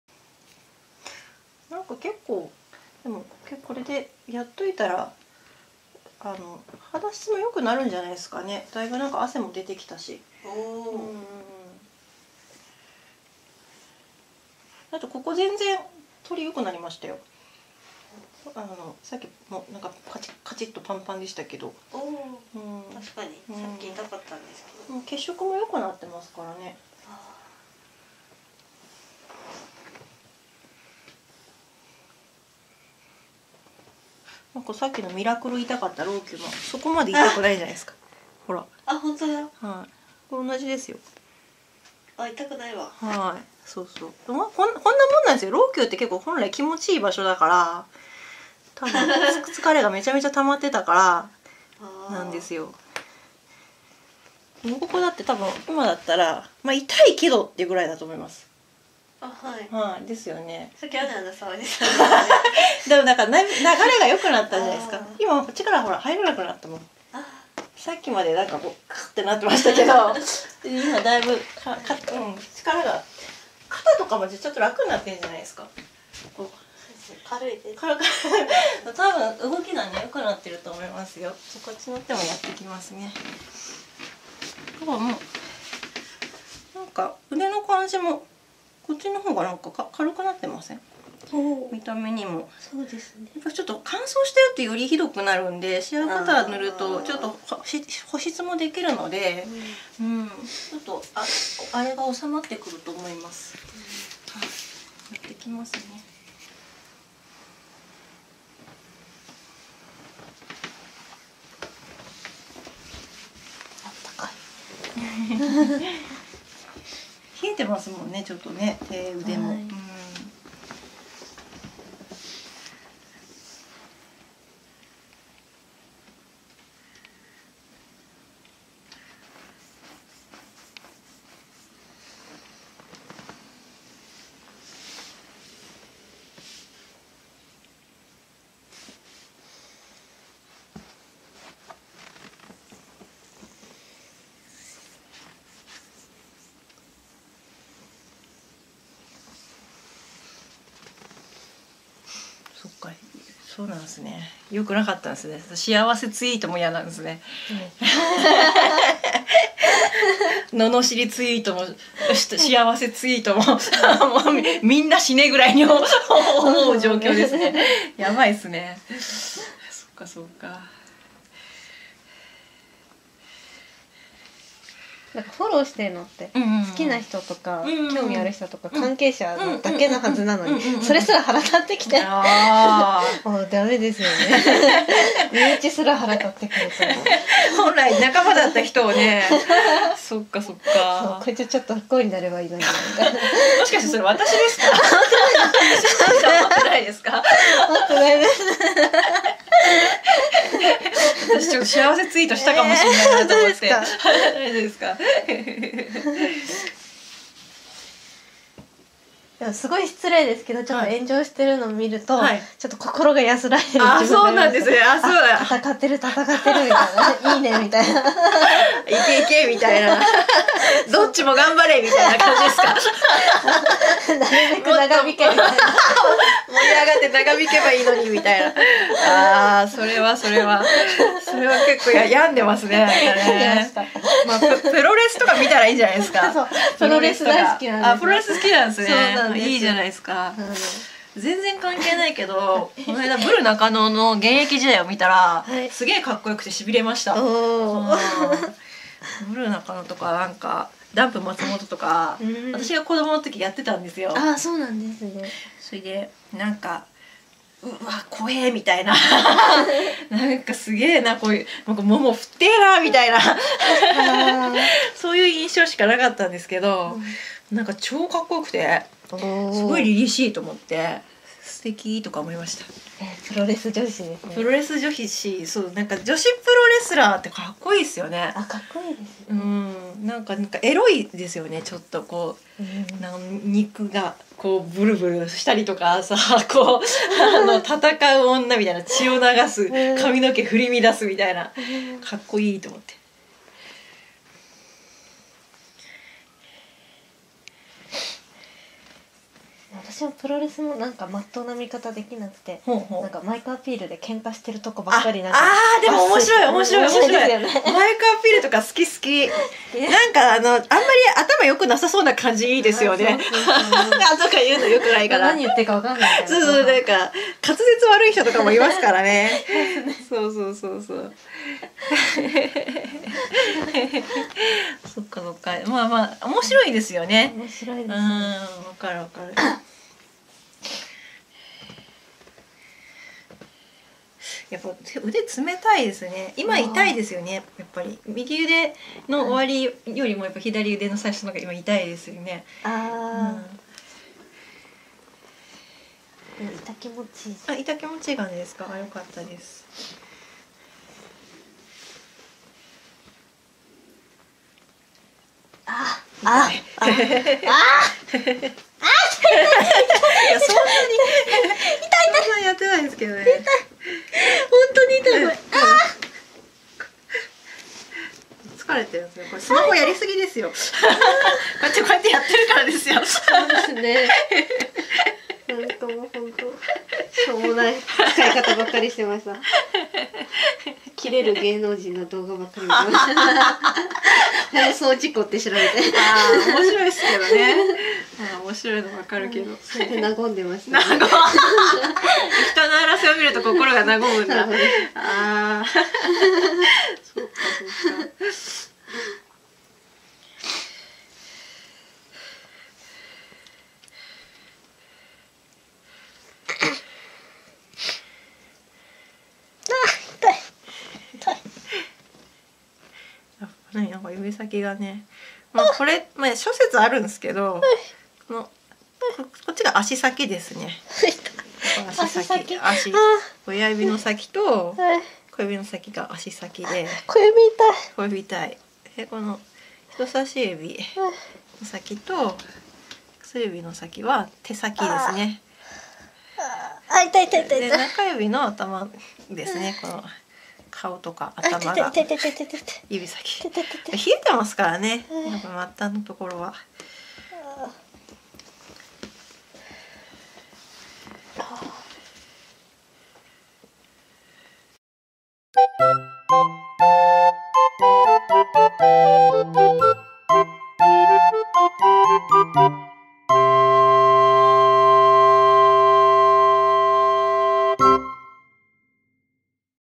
【ももは】２５歳女性にハンドリフレ３【りらく屋】
ももはさんにシアバターで手腕のトリートメント。雑談多めです。